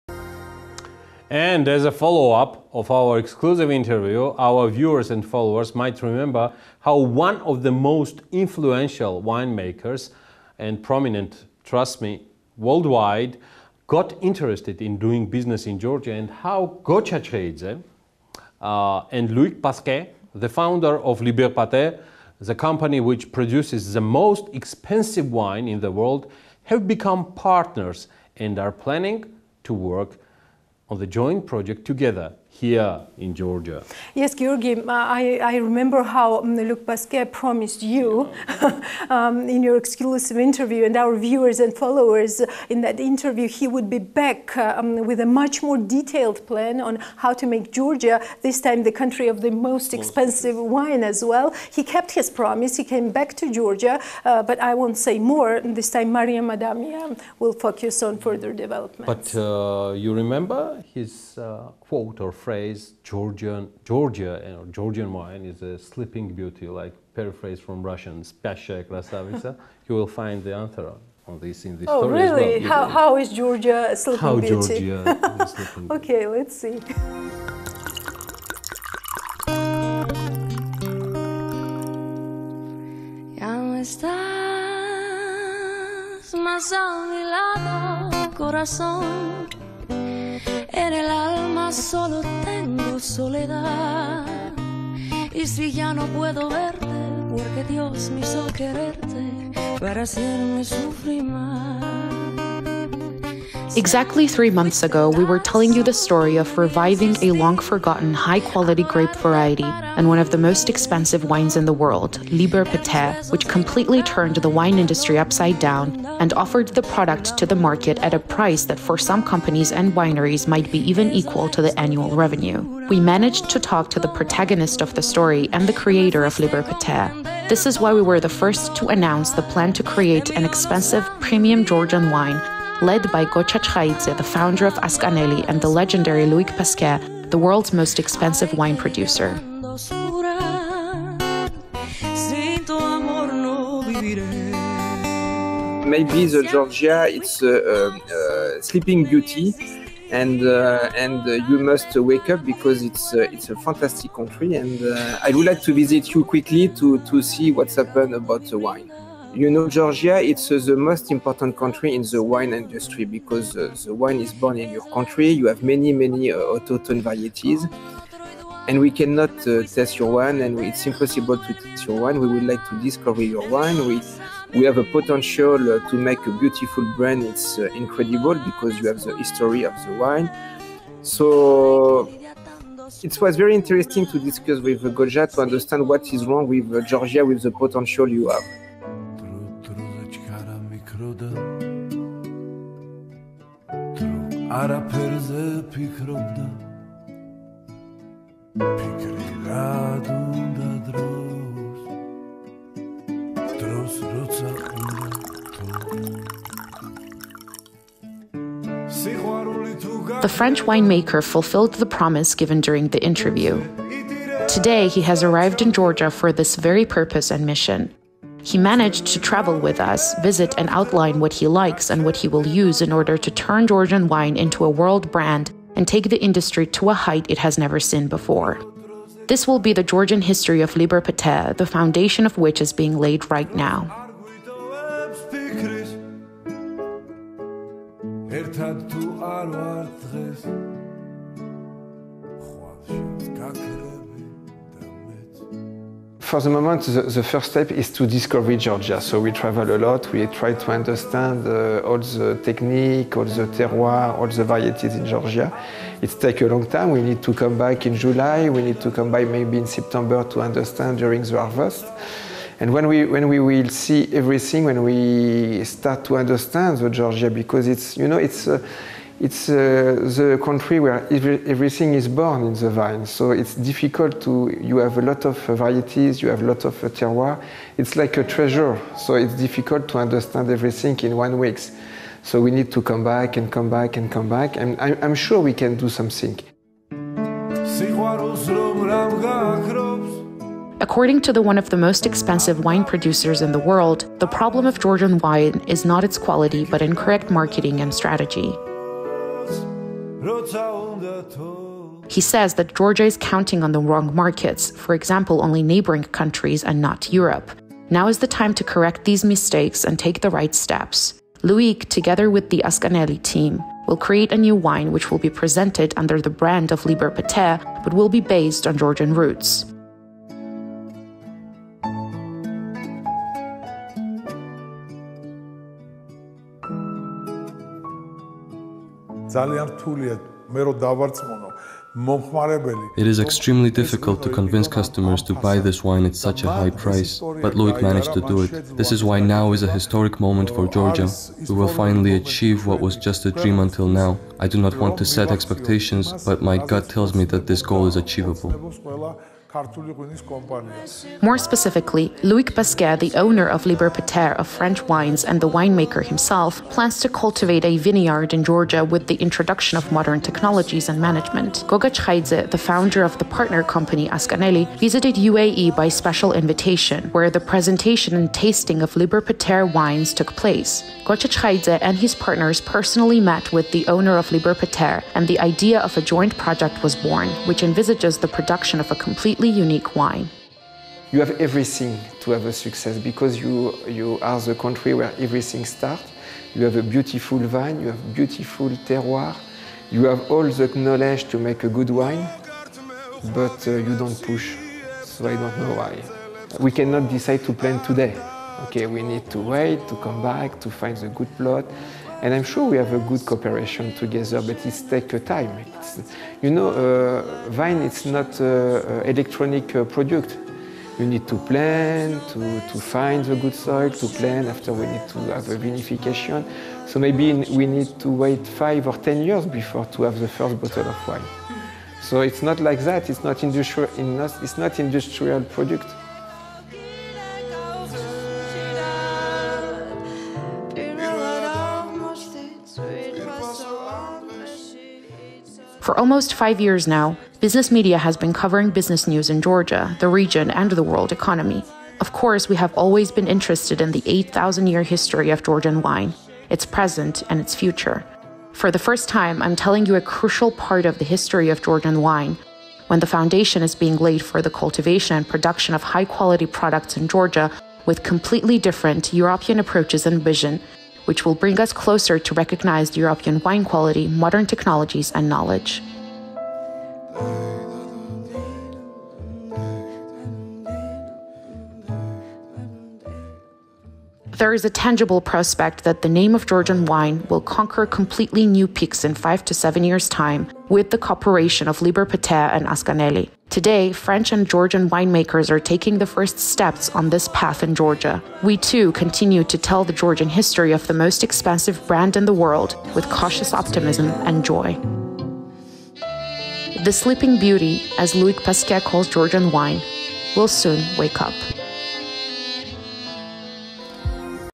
Loic Pasquet returns to Georgia
And as a follow-up on my exclusive interview, our viewers and followers might remember how one of the most influential winemakers in the world got interested in doing business in Georgia and how Gocha Chkhaidze and Loic Pasquet, the founder of "Liber Pater", the company which produces the most expensive wine in the world, have become partners and are planning to work on the joint project together.
Yes, Giorgi. I remember how Loic Pasquet promised you and our viewers and followers in that interview, he would be back with a much more detailed plan on how to make Georgia - this time the country of the most expensive wine. He kept his promise. He came back to Georgia. But, I won’t say more - this time Mariam Adamia will focus on further developments.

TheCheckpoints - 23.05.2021